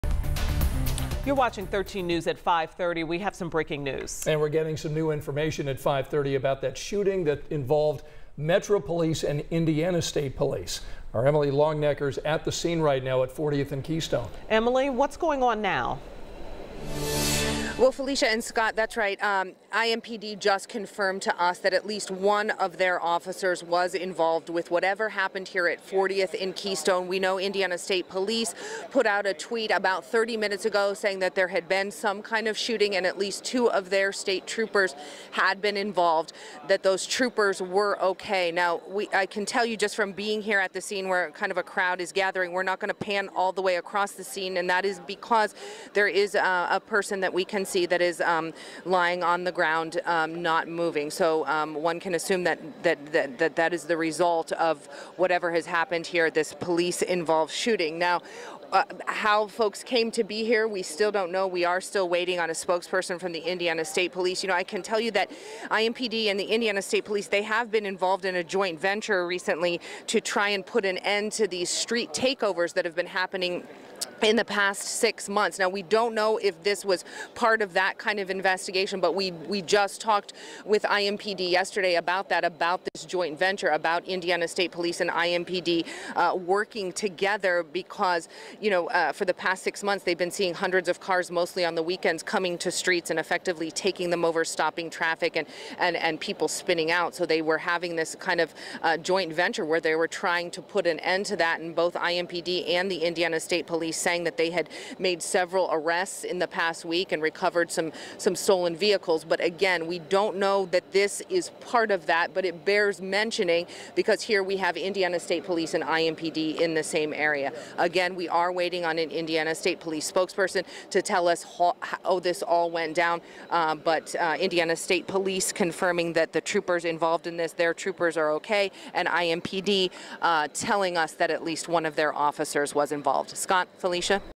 Police investigate officer-involved shooting on Keystone Avenue
No police officers were injured in the incident, authorities said.